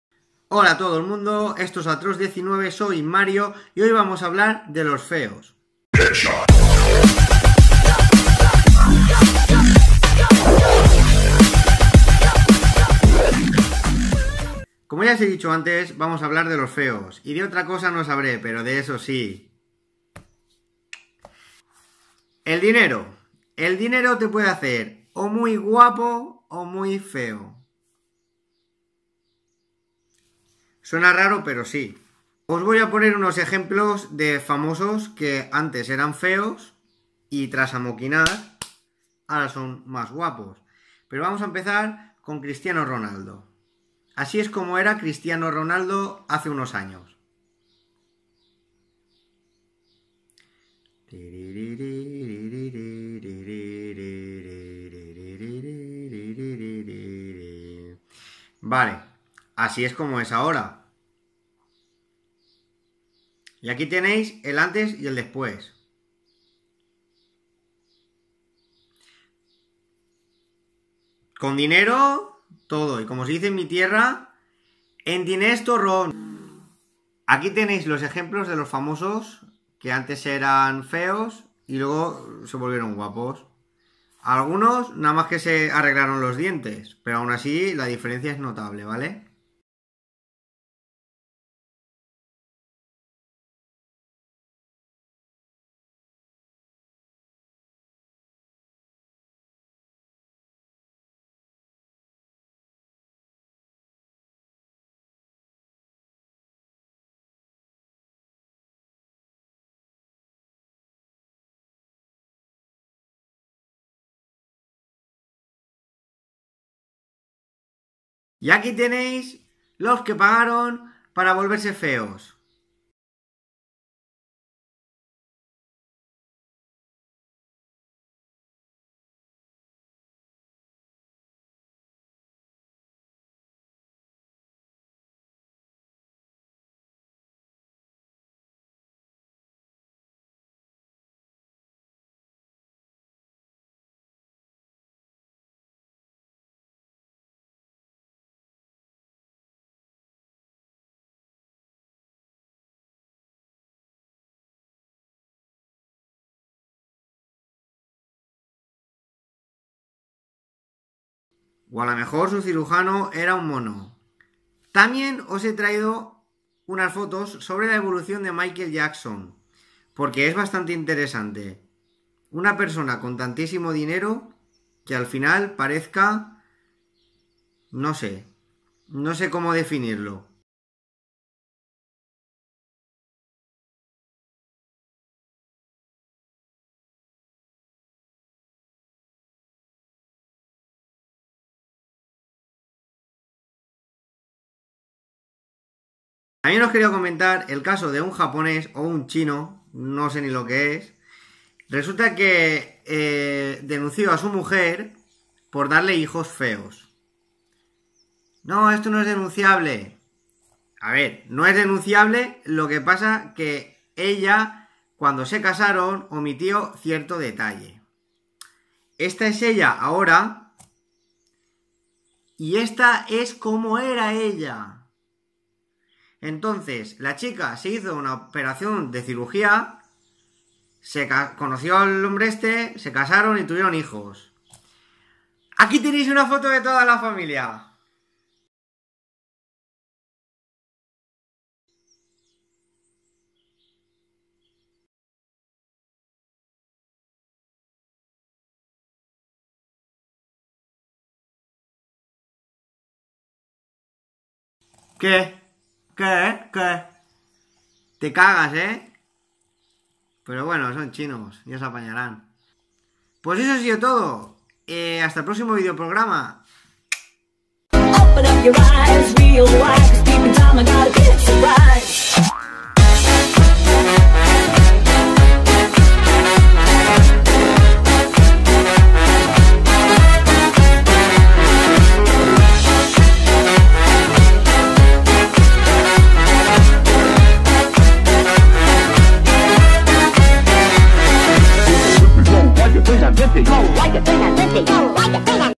Feos y guapos
Gente que paga para ser guapa y otros para ser feos